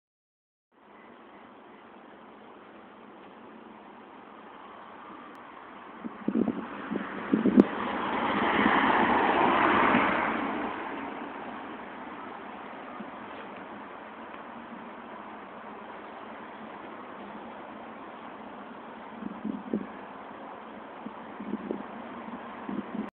Smashing Car's Window Vandalism in San Francisco
Car's smashed window in the middle of a sunday noon in the Golden Gate Park.Smashing windows to steal from inside the cars is becomimg frequent in San Francisco.